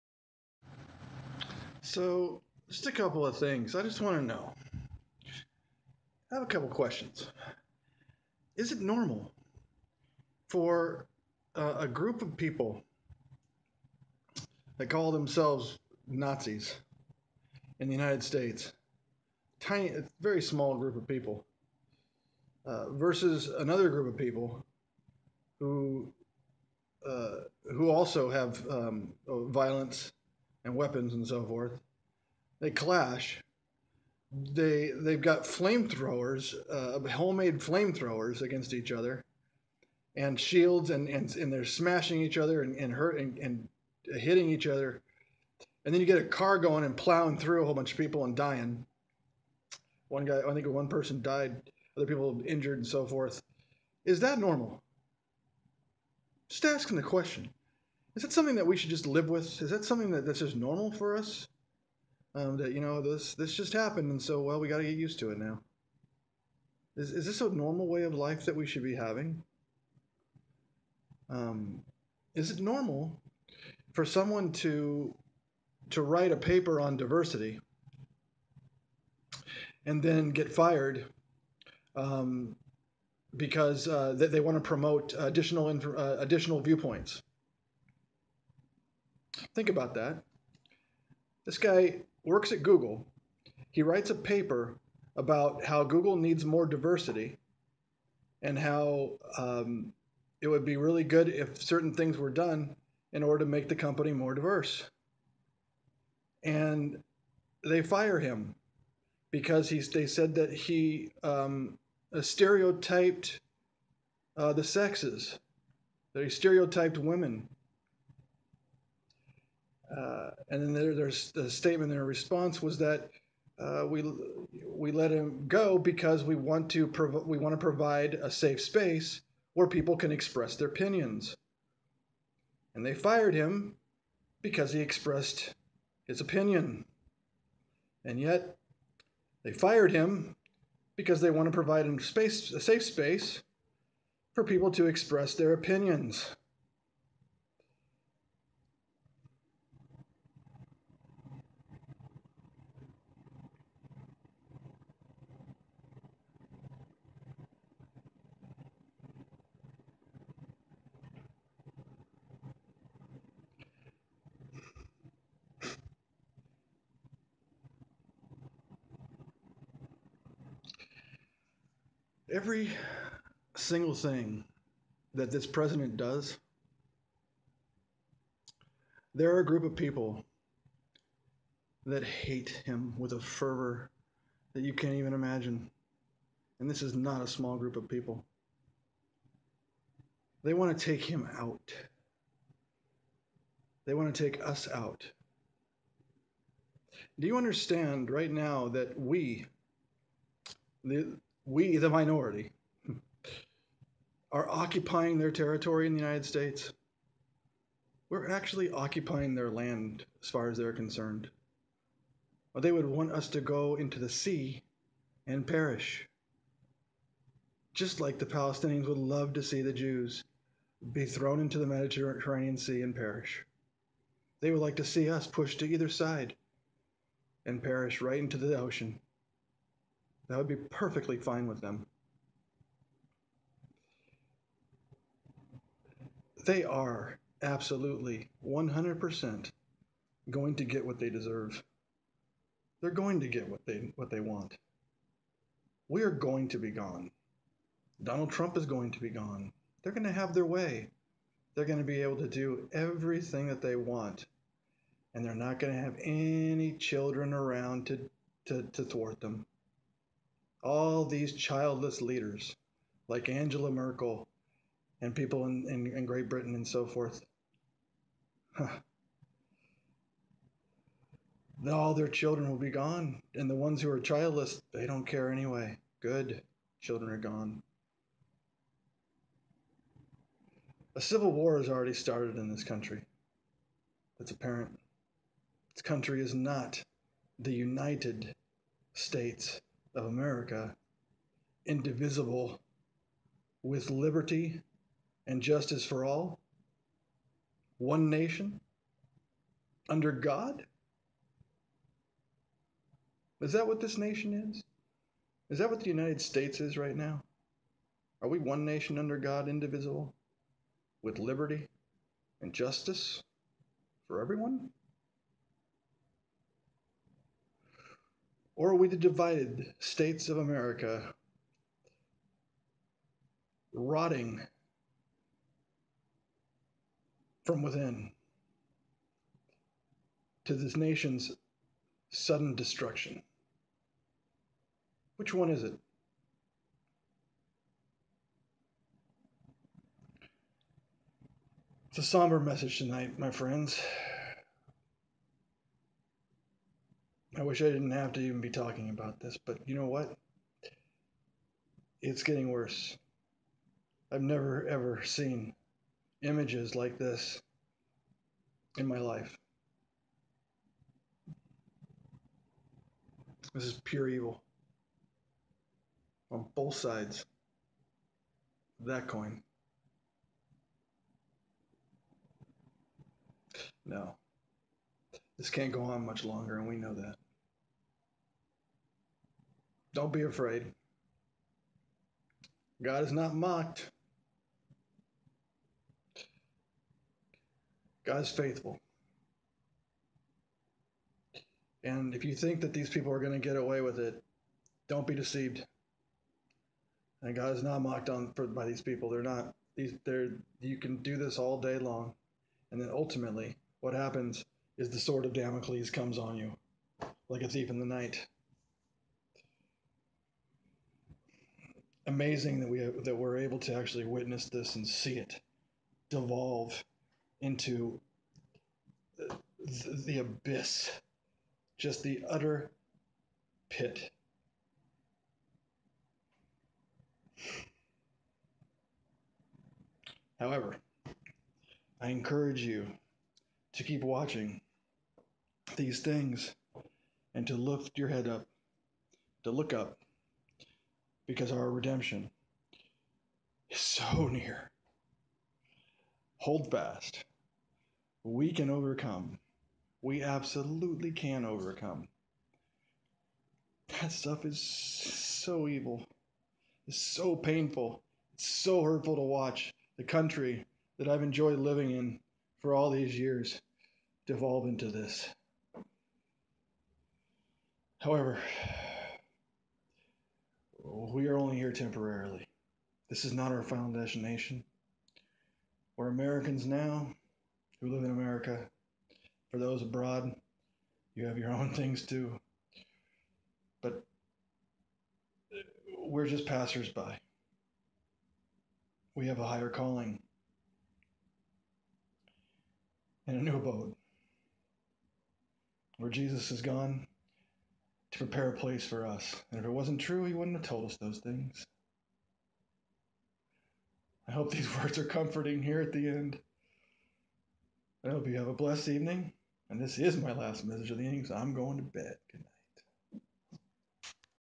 Revelation 12 Sign - One Nation Indivisible?
What is going on in the US?

Simple. This last weekend, we witnessed the official beginning of America's Last Civil War in Charlottesville, VA, just over a week before the eclipse to repentance.